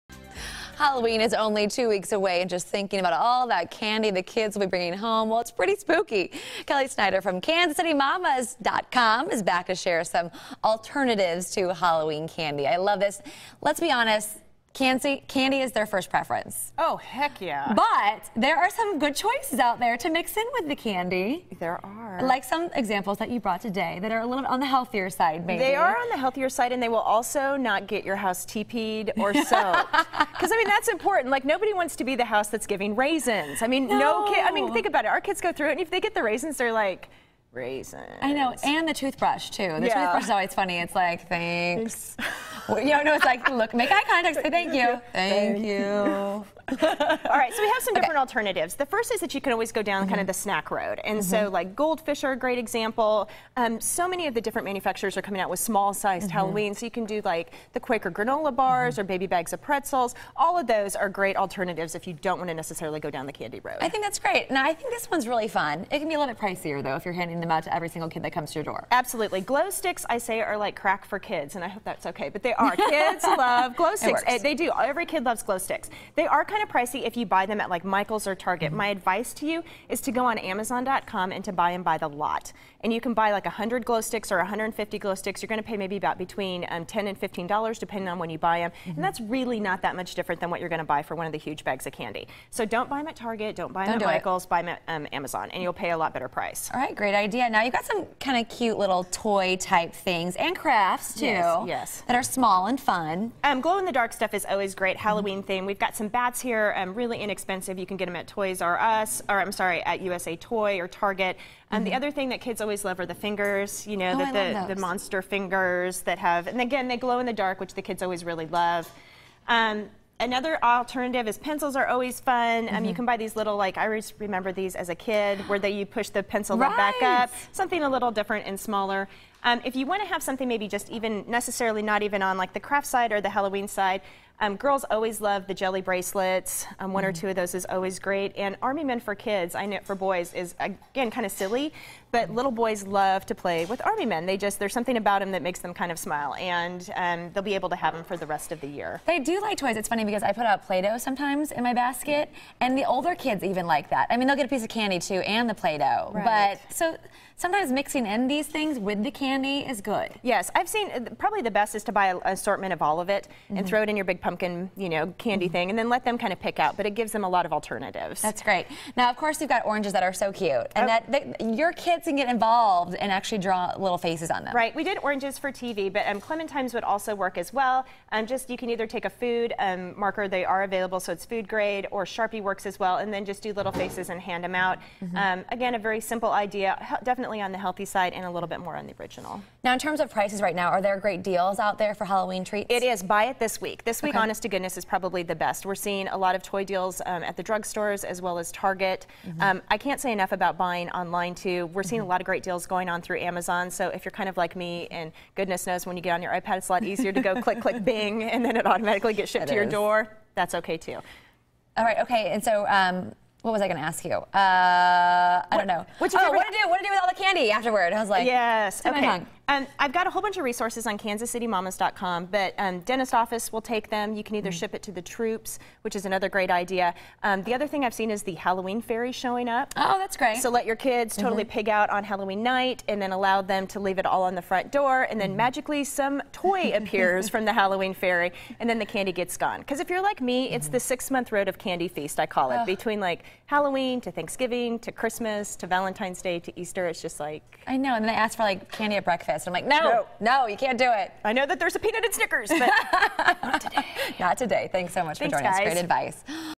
KCL - Halloween candy alternatives that will keep kids coming back
Are you looking for some alternatives to candy this Halloween? Kelly Snyder from KansasCityMamas.com shares some alternatives that will be sure to keep kids coming back!